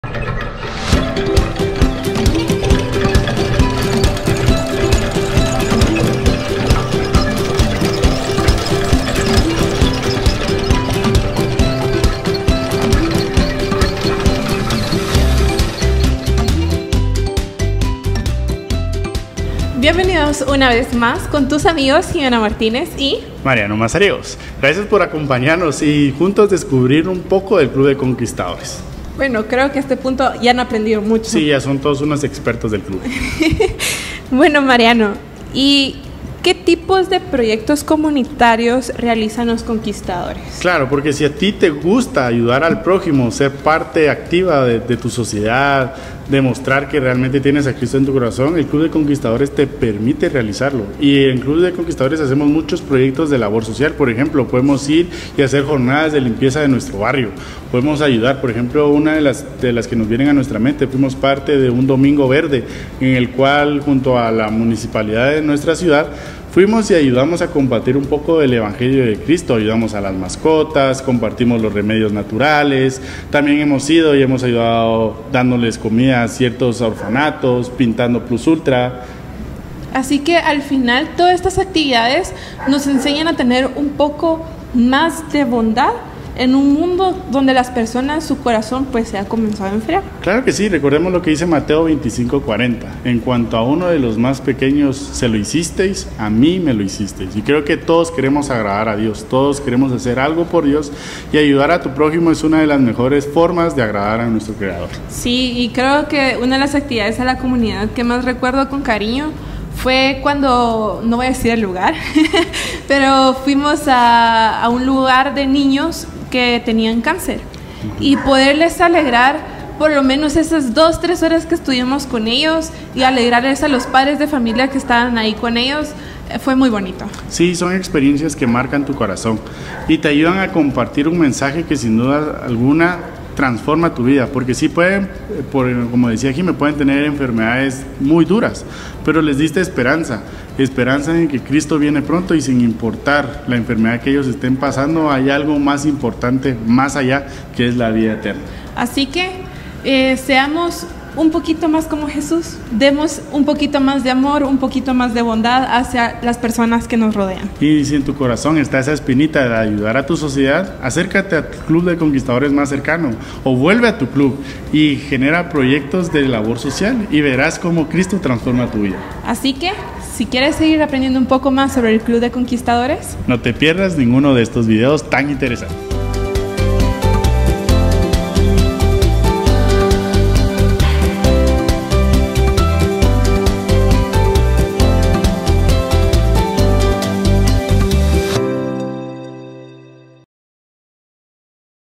CLUB DE CONQUISTADORES - PROYECTOS COMUNITARIOS
💪🌍 Proyectos Comunitarios de los Conquistadores 🌍💪

Los Conquistadores no solo aprenden, ¡también actúan! 🙌 Desde campañas de limpieza ambiental hasta ayudar a personas necesitadas, el servicio es una parte fundamental de la misión 💖.

 ¡Cada esfuerzo cuenta y hace una diferencia real en la vida de las personas! 🌟🤝